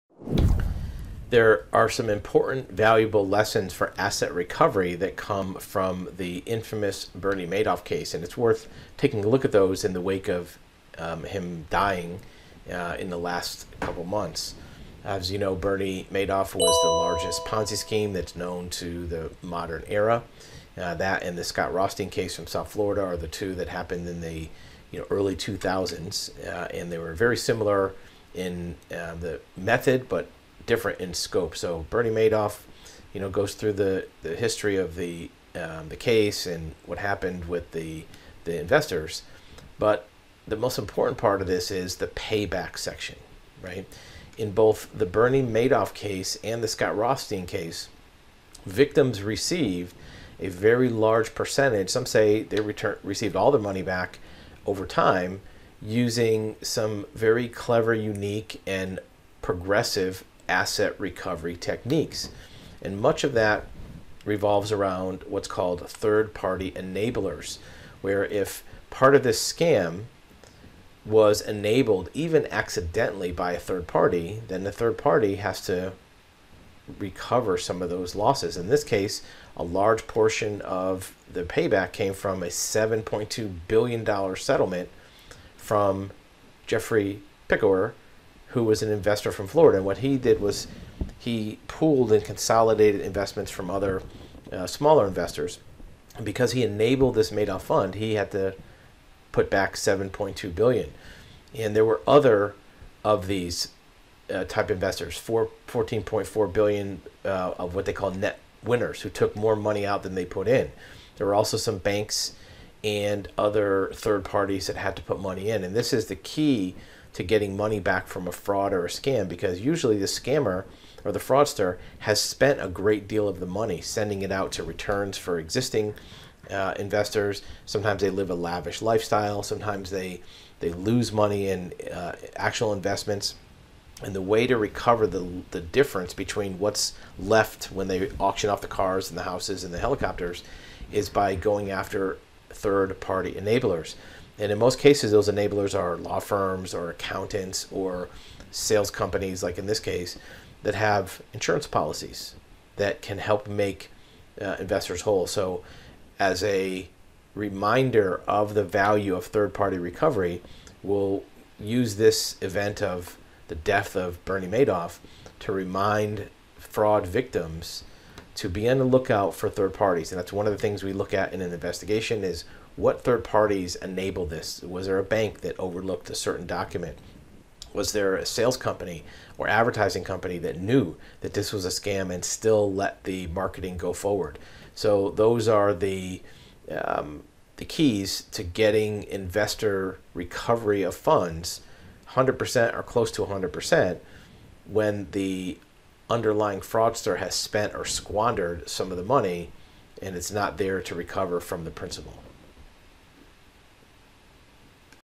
Asset Recovery Success Lessons From The Madoff Case
How investor victims get most or all of their money back using constructive trustee third party liability